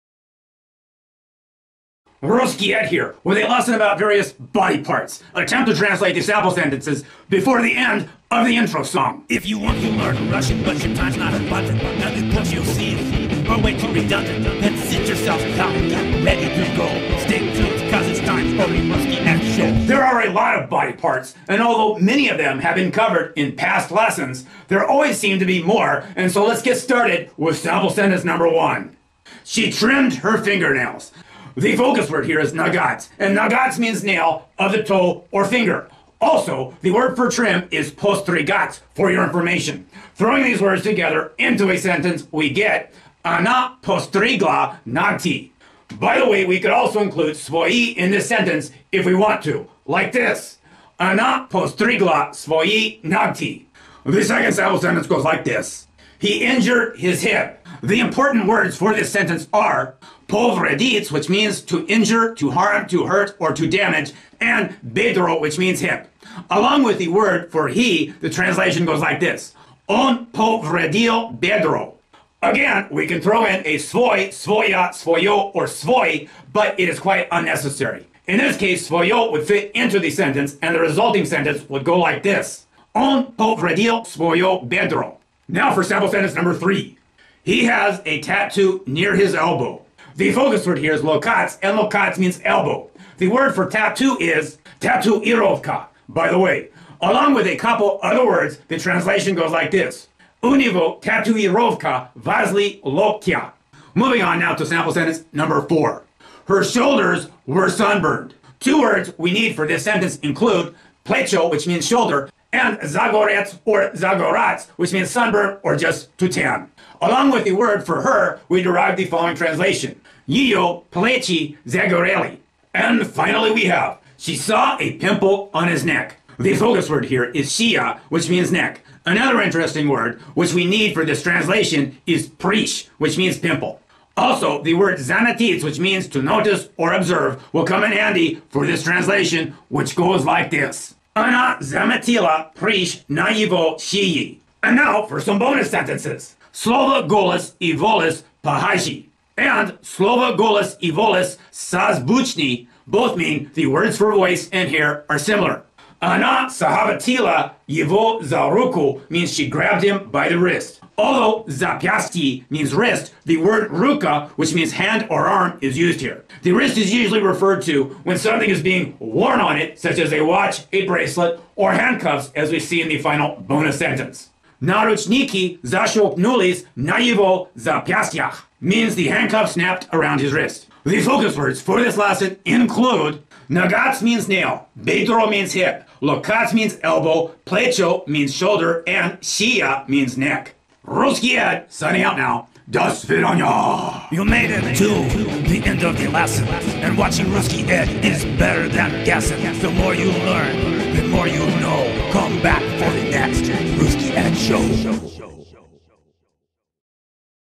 English to Russian 19 - бедро, локоть, плечо
She trimmed her fingernails.
He injured his hip.
He has a tattoo near his elbow.
Her shoulders were sunburned.
She saw a pimple on his neck.

TRANSCRIPT:
Rusky Ed here with a lesson about various body parts. Attempt to translate the sample sentences before the end of the intro song.

There are a lot of body parts and although many of them have been covered in past lessons, there always seem to be more and so let's get started with sample sentence number one:

The focus word here is ноготь and ноготь means nail of the toe or finger. Also the word for trim is постригать, FYI. Throwing these words together into a sentence we get.
Она постригла ногти.
BTW, we could also include свои in this sentence if we want to, like this:
Она постригла свои ногти.

The second sample sentence goes like this:
The important words for this sentences are:
повредить - injure, harm, hurt, damage
бедро - hip
Along with the word for he the translation goes like this:
Он повредил бедро.
Again we can throw in a свой, своя, своё, or свои, but it is quite unnecessary. In this case своё would fit into the sentence and the resulting sentence would go like this:
Он повредил своё бедро.

Now for sample sentence three:
The focus word here is локоть and локоть  means elbow. The word for tattoo is татуировка, BTW. Along with a couple other words the translation goes like this:
У него татуировка возле локтя.

Moving on now to sample sentence number four:
Two words we need for this sentence include:
плечо - shoulder
загореть/загорать - sunburn (or just to tan)
Along with the word for her we derive the following translation:
Её плечи загорели.

And finally we have:
The focus word here is шея which means neck. Another interesting word which we need for this translation is прыщ which means pimple. Also the word заметить which means to notice or observe will come in handy for this translation which goes like this:
Она заметила прыщ на его шее.

And now for some bonus sentences:
Слова голос и волос похожи?
Слова голос и волос созвучны?
(The words for voice and hair are similar.)
Она схватила его за руку.
(She grabbed him by the wrist.)
Although запястье means wrist, the word рука which means hand or arm is used here. The wrist is usually referred to when something is being worn on it such as a watch, a bracelet, or handcuffs as we see in the final bonus sentence.
Наручники защёлкнулись на его запястьях.
(The handcuffs snapped around his wrists.)

The focus words for this lesson include:
ноготь (nail)
бедро (hip)
локоть (elbow)
плечо (shoulder)
шея (neck)